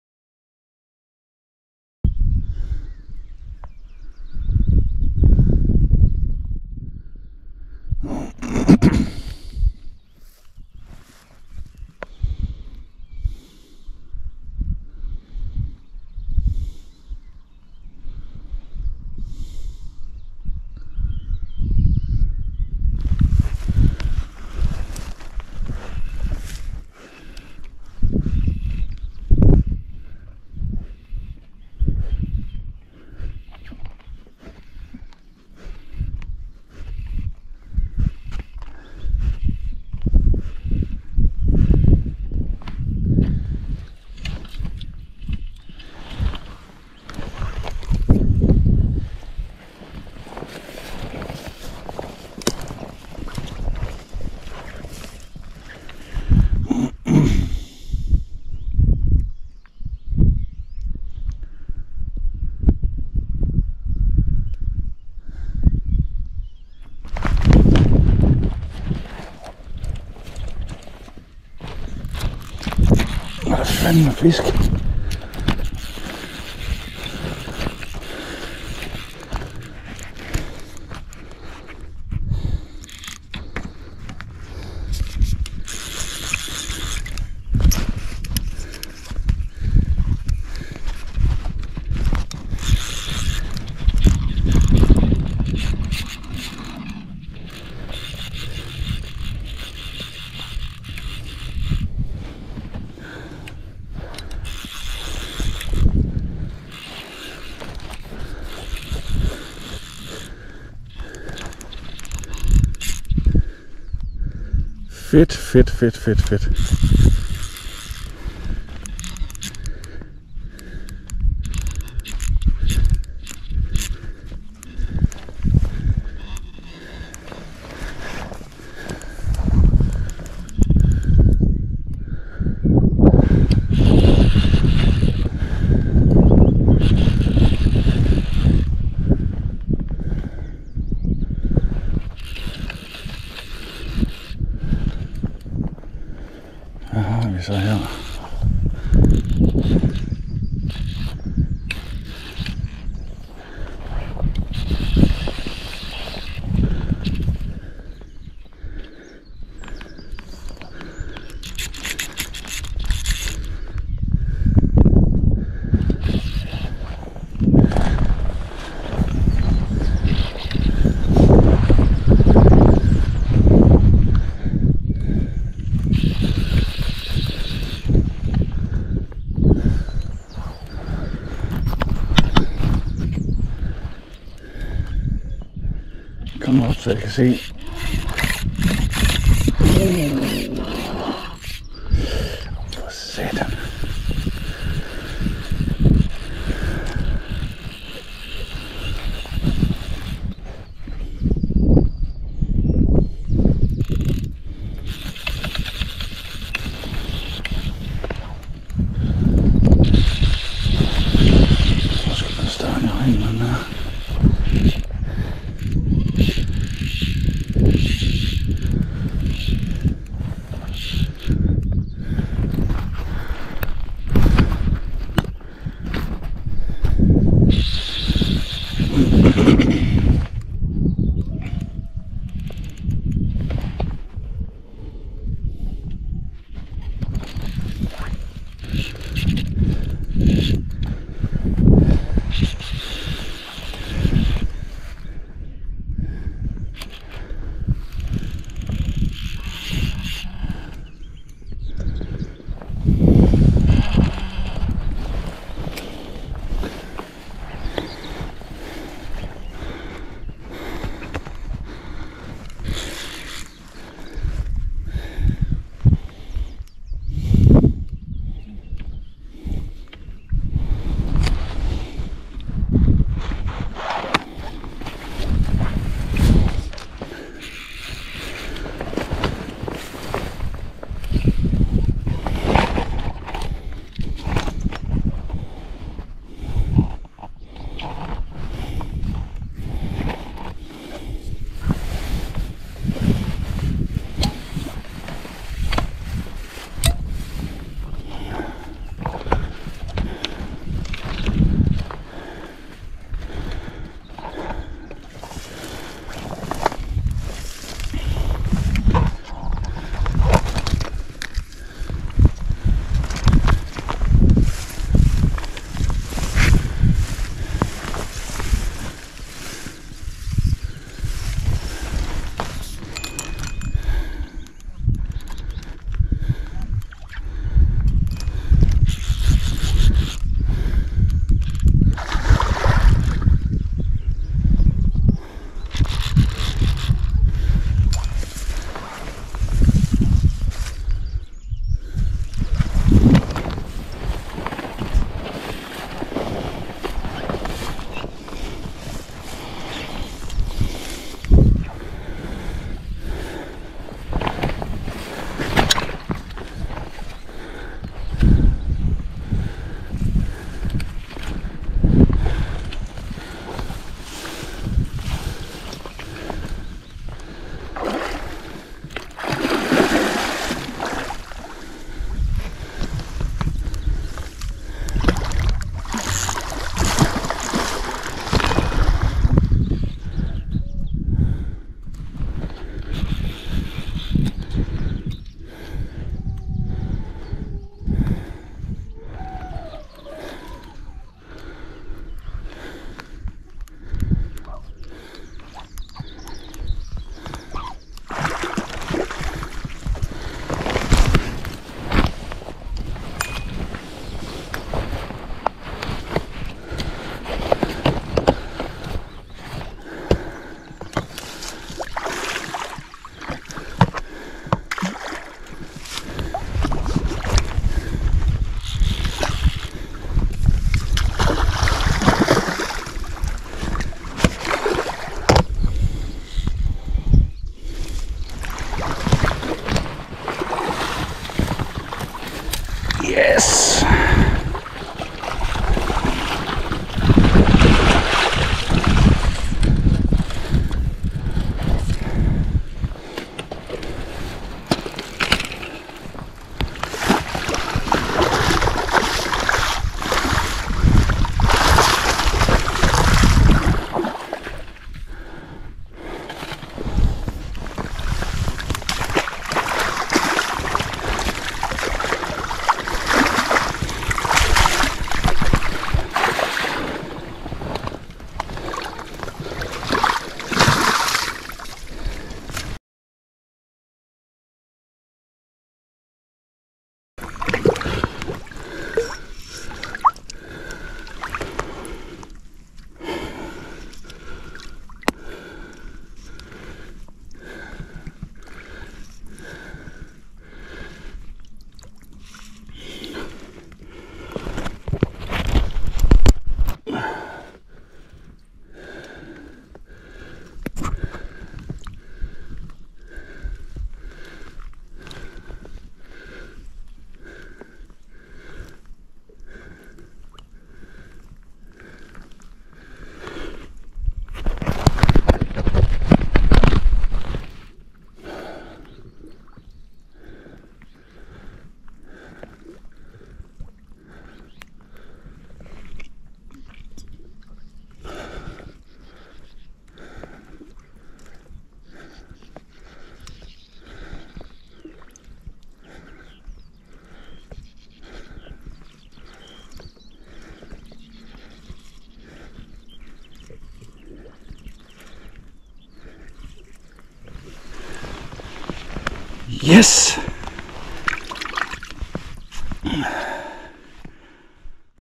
My GoPro Video
Efter mange ture var der endelig en storålaks til mig og endda på det lette grej
Guideline ULS hybrid klasse 7 klarede opgaven perfekt
Flot flot laks på 95 cm tog min røde Plug francis efter 15-20 min fiskeri og  kunne  nænsomt genudsættes 🥰🥰